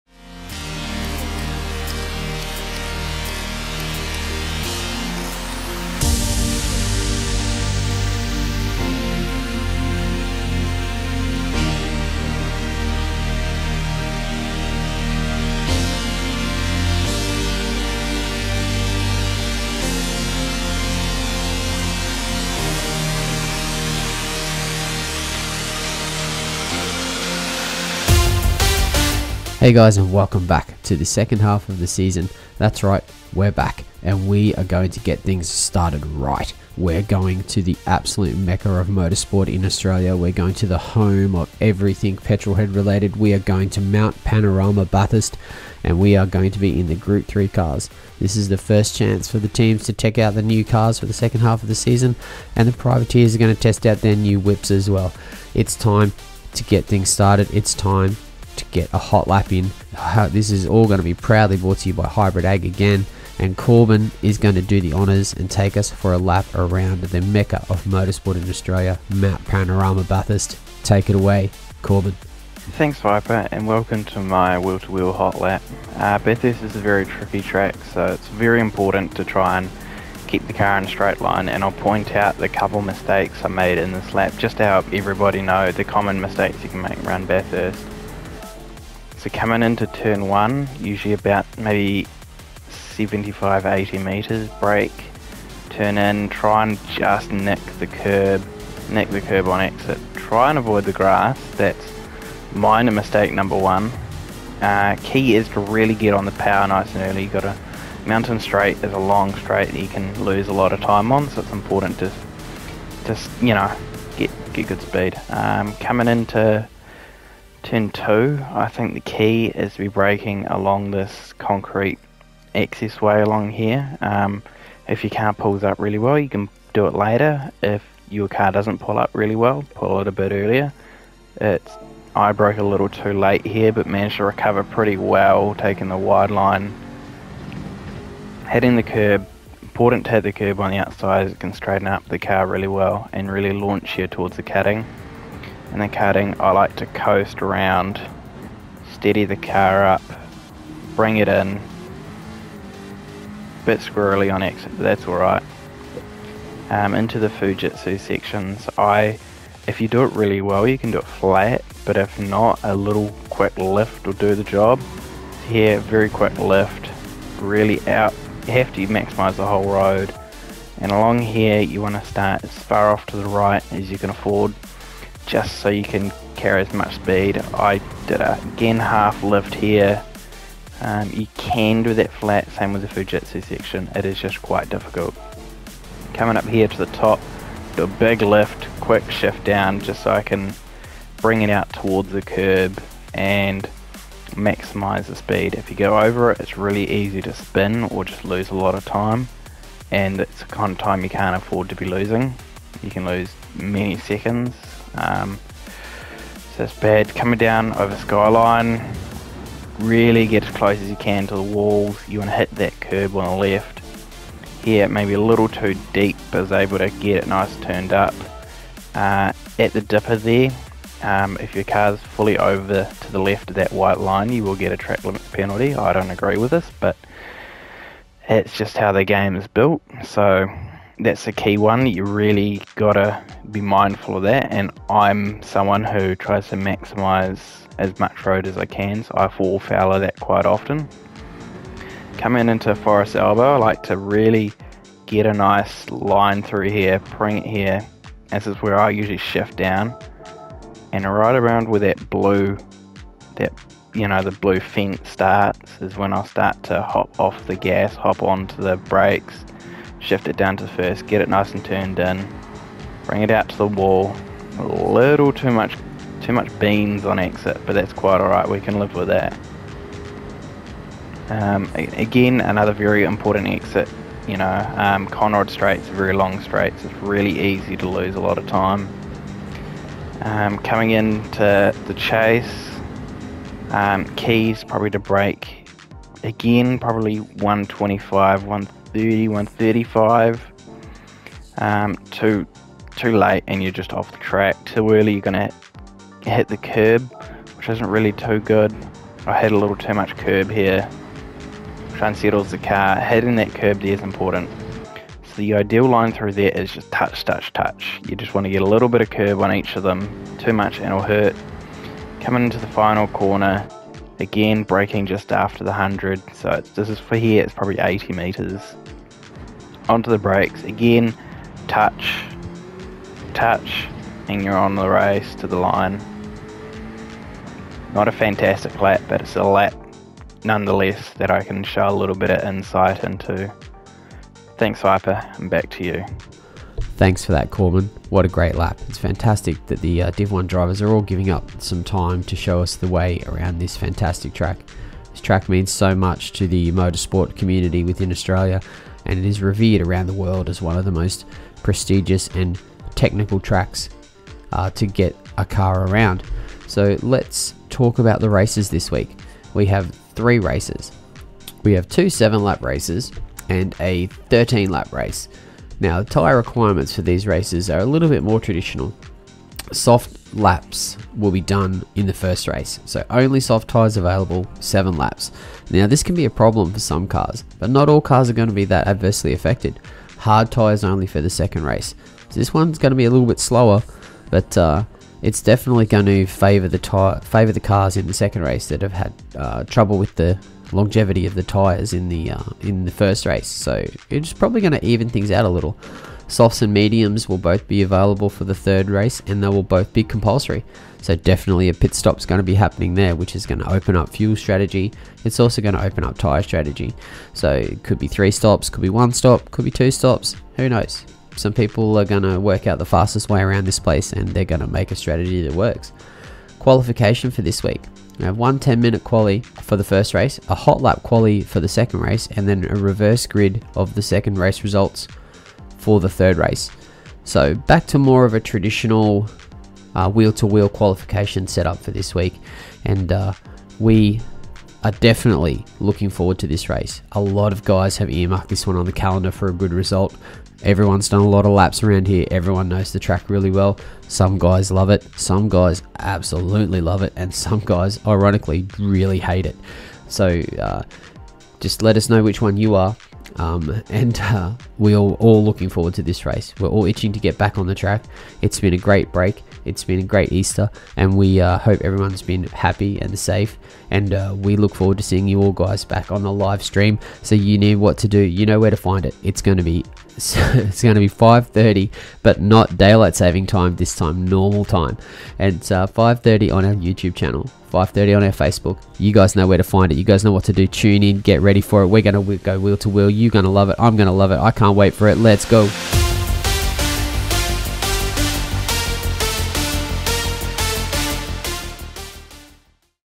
W2W RACING SEASON 13 / GR3 Manufacturer / Bathurst Hotlap
We're back baby!. Join Viper as he previews what is to come for the first round after our 2 week break. This week we’re tackling Bathurst, a place that Division 1 Driver CCH knows particularly well. Jump on board for a wild ride with him around the mountain!

These hotlaps are all thanks to Hybrid Ag, check them out today